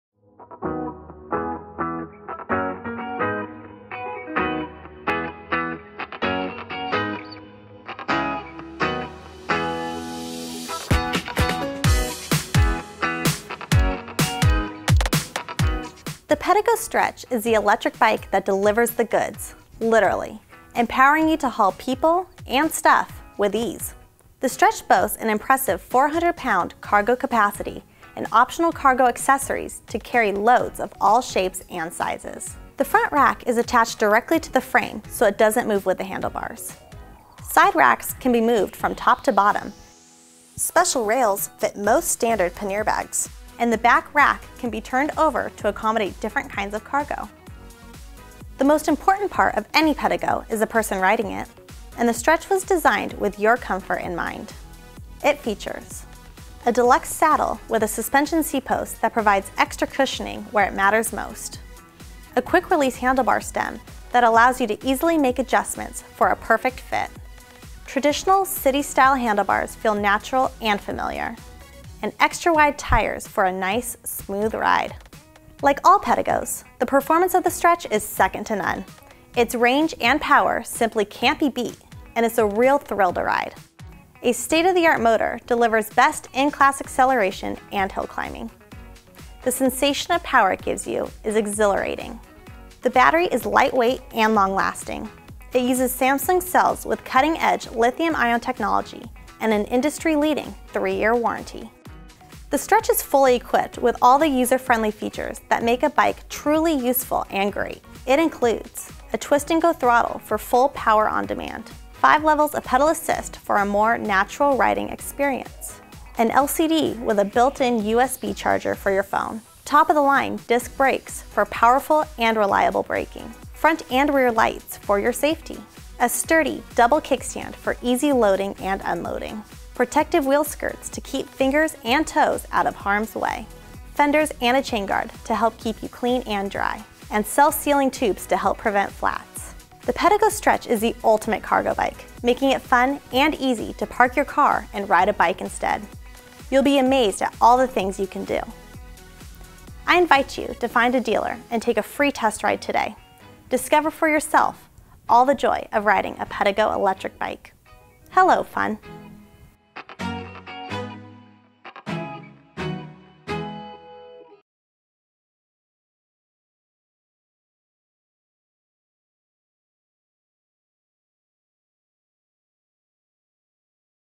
The Pedego Stretch Electric Bicycle At Electric Vehicle Mall
The fabulous Pedego Stretch is the electric bike that delivers the goods (literally) — empowering you to haul people and stuff with ease! FEATURES include: 400 pound cargo capacity, adjustable side  and back rack for different kinds of cargo, protective wheelskirts, five levels of pedal assist, extra wide tires and much much more. We're open 10am to 5:30 pm M-F and 11am to 2 pm on Saturday, EST. Buy this electric bike and enhance your life!